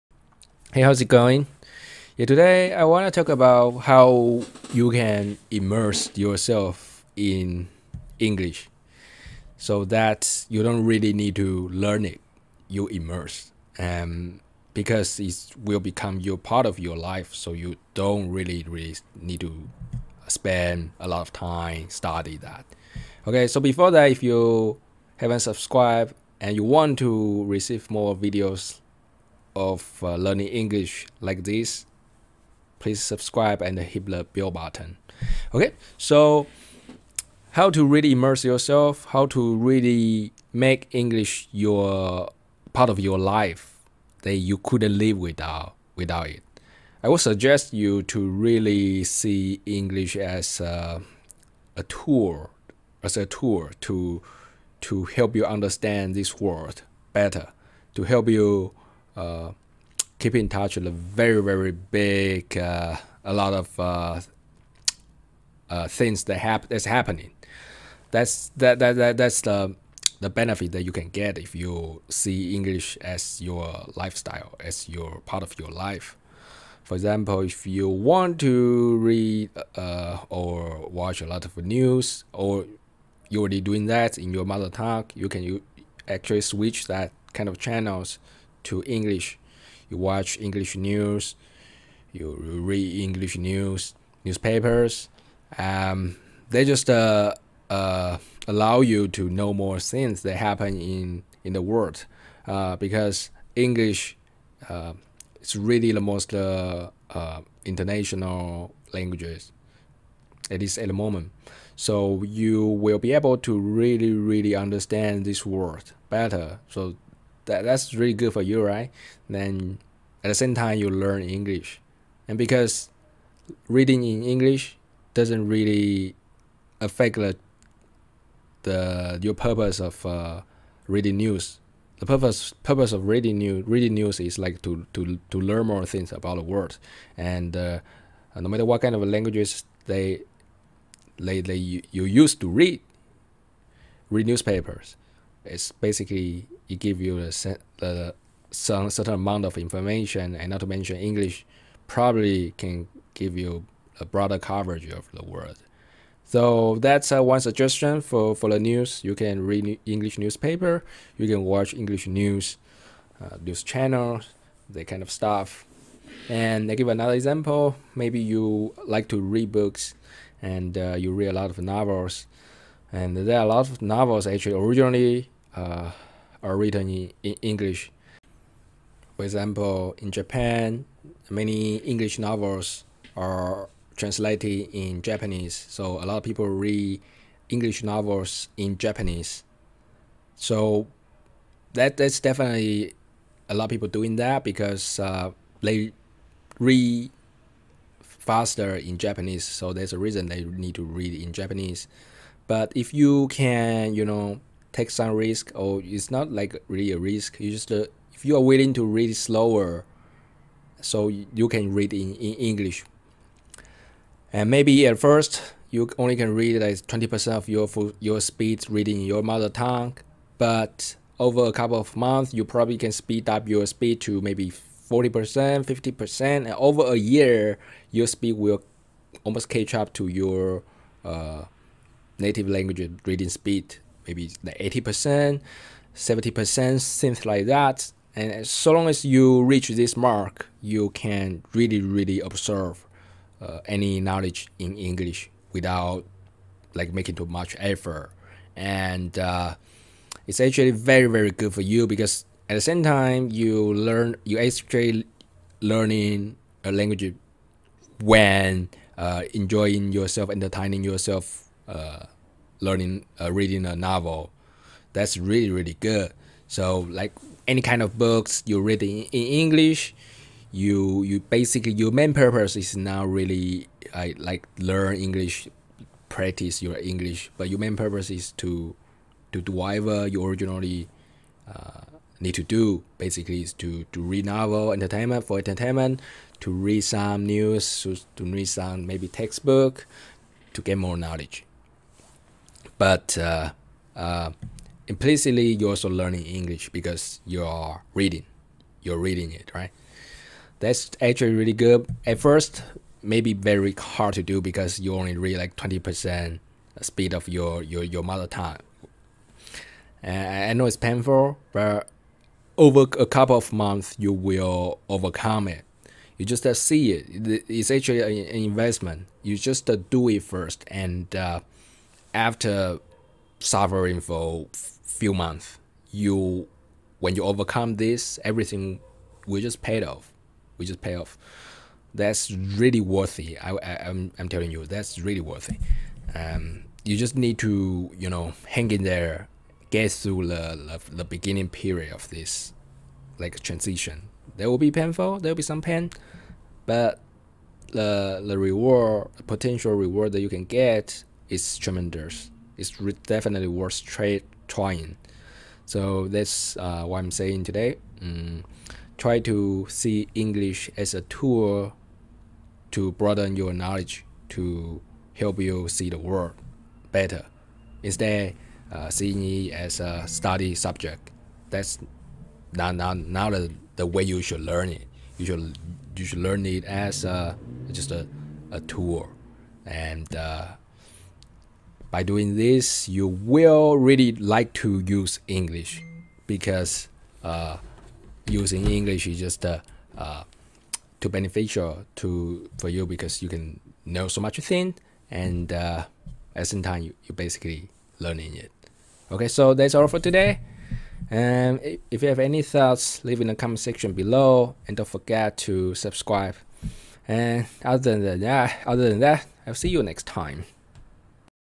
How to Immerse Yourself in English
Wouldn't it be nice if you can do things you love and learning English at the same time? This video gives some tips on how you can actually achieve it.